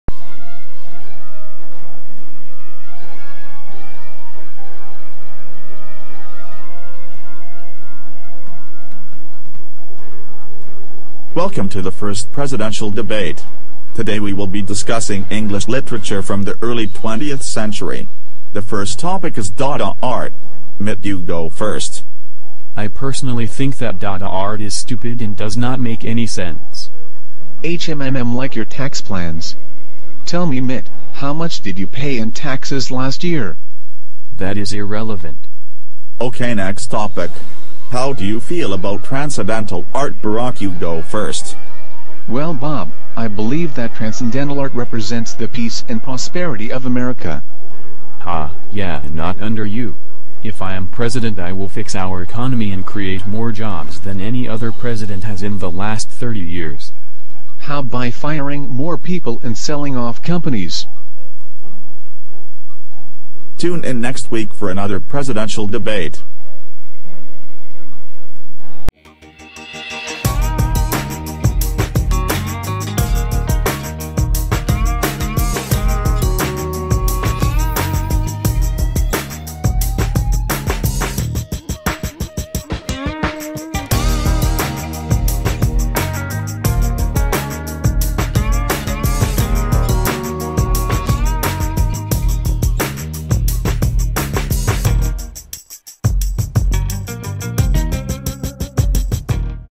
Worst Video Ever Made
Ever wonder what it would be like if Barack Obama and Mitt Romney talked about something other than politics? Watch and find out what heppens!

I made this video for my English class about what we studied the first half of the year. Hope you enjoy!

If you enjoyed a rating would be much appreciated :)